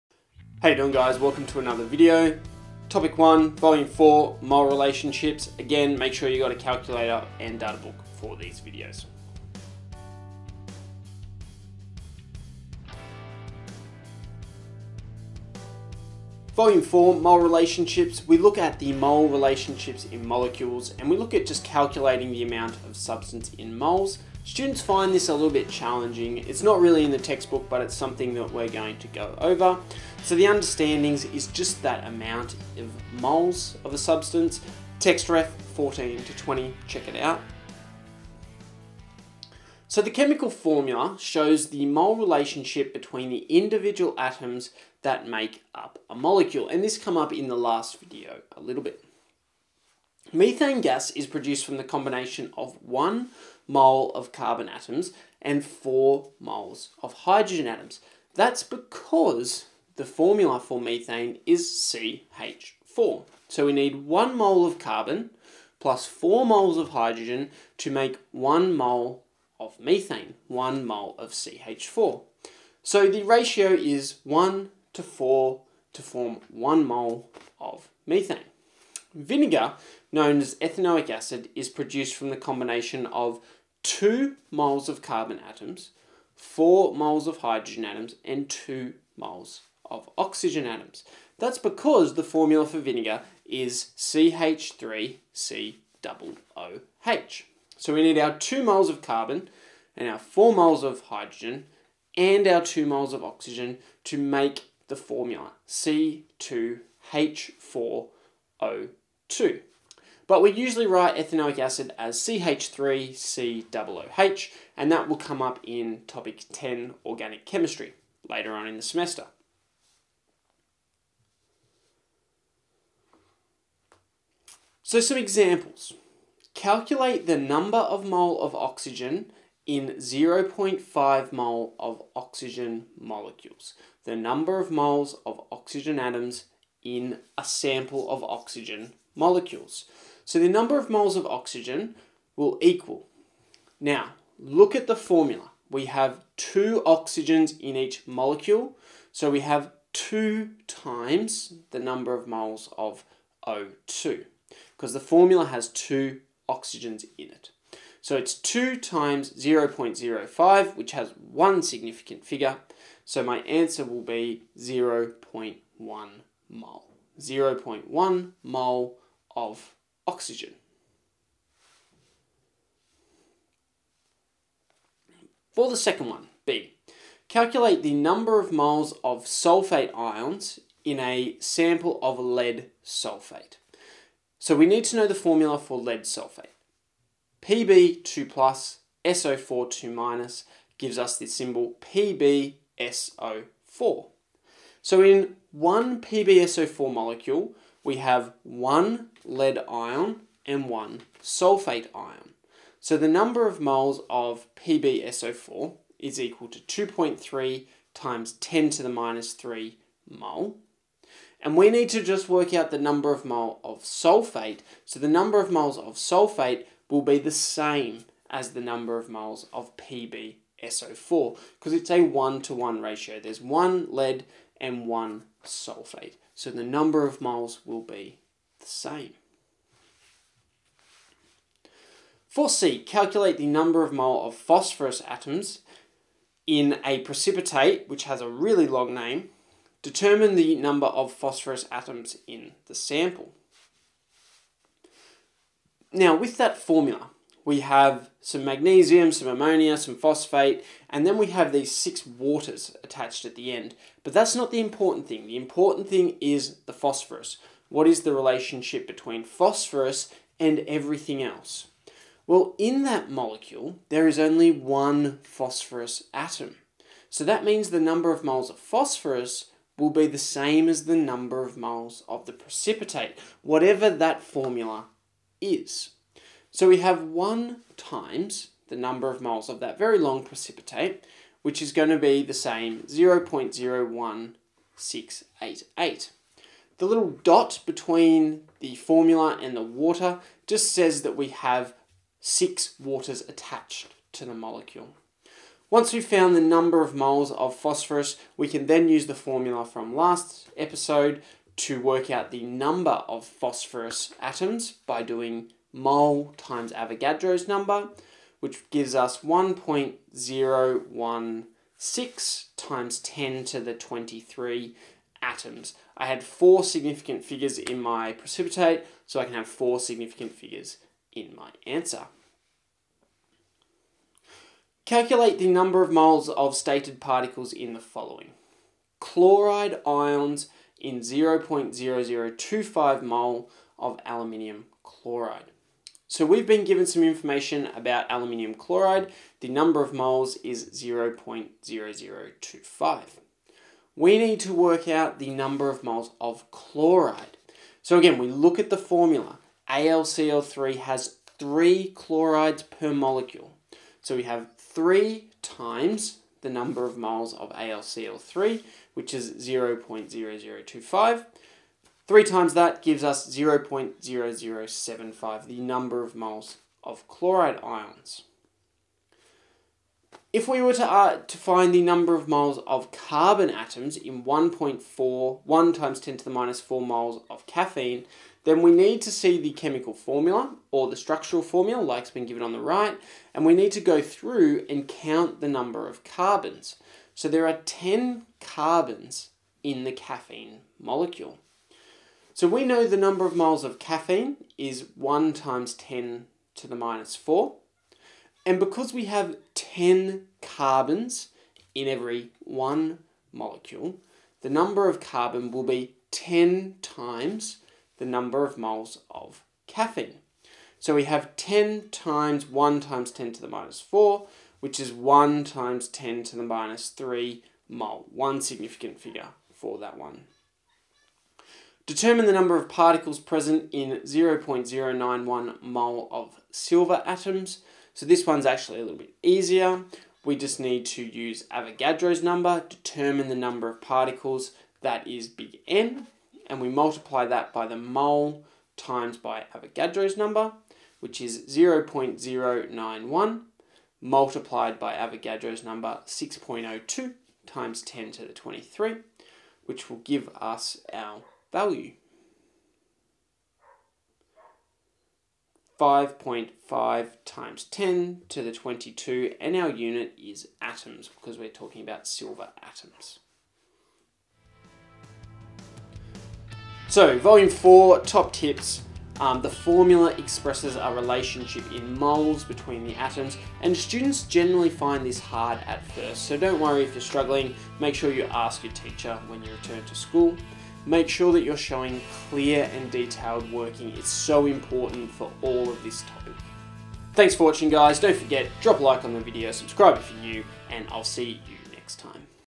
Topic 1. 2 Volume 4 - Mole relationships
IB Chemistry  Topic 1. 2 Volume 4 - Mole relationships
- Mole relationships in molecules
- Calculating the amount of substance in mol